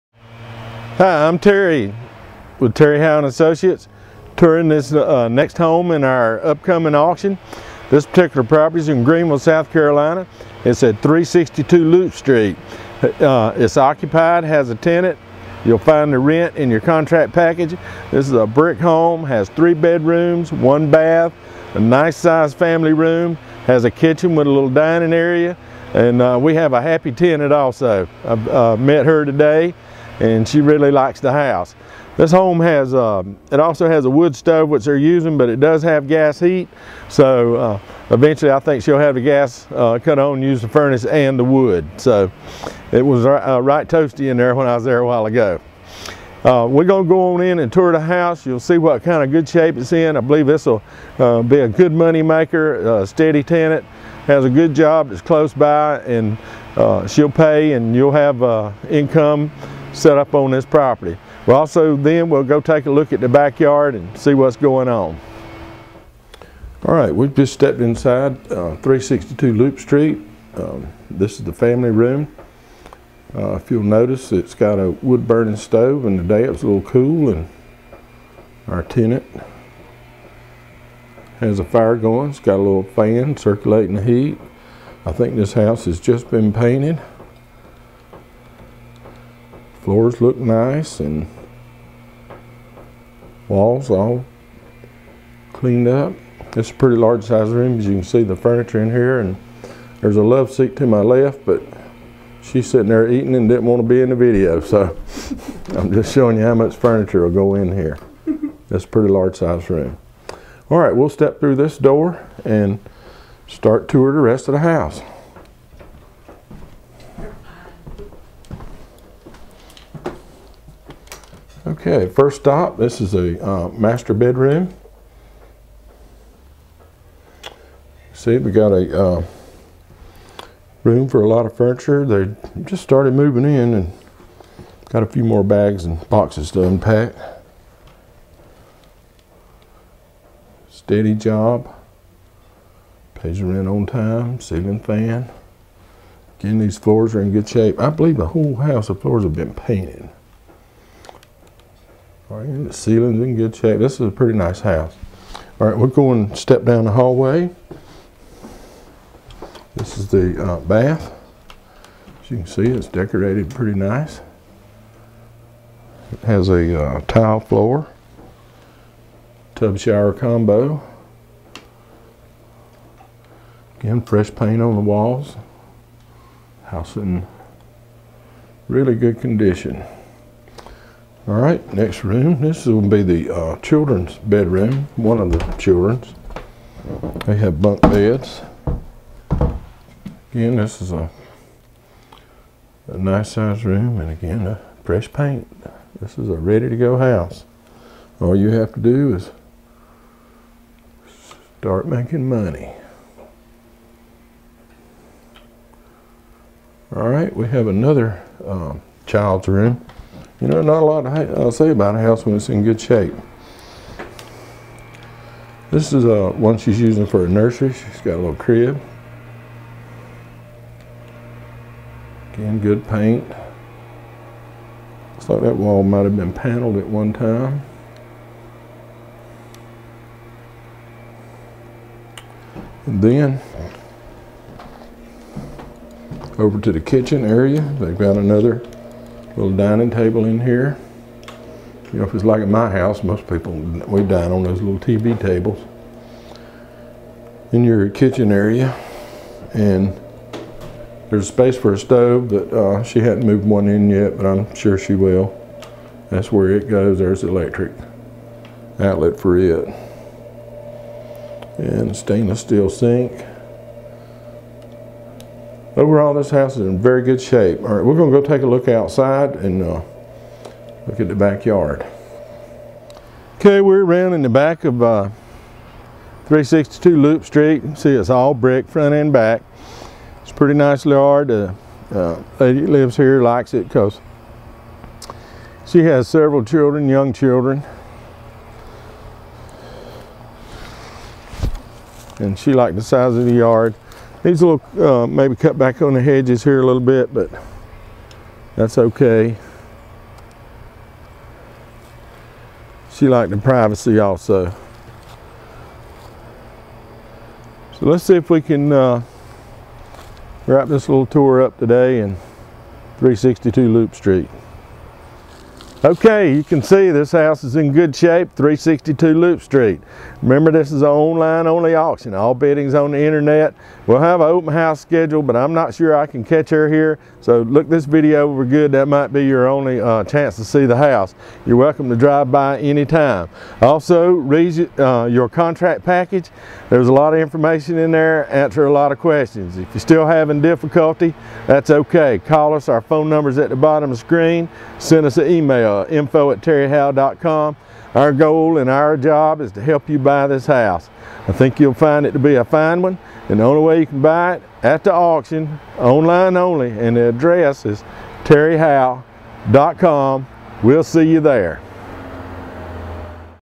362 Loop St, Greenville, SC 29609 - Online Only Real Estate Auction
This is a brick home with 3 bedrooms and 1 bathroom. There is a living room with fireplace and a kitchen with a small dining area. It is located between Poinsett Hwy and Rutherford Rd.

This home is currently rented.

More information, including property video and Contract Package, is coming soon.

This seller of this home is offering 14 properties in this month's auction, including 13 houses and 1 lot. The seller has guaranteed to sell at least 6 of the homes regardless of price. It is the seller's goal to sell all of the properties.

The properties listed as FR01 through FR14 are included in this list. All of the properties are in Greenville, SC. Some of the properties are currently rented. Some are rent ready.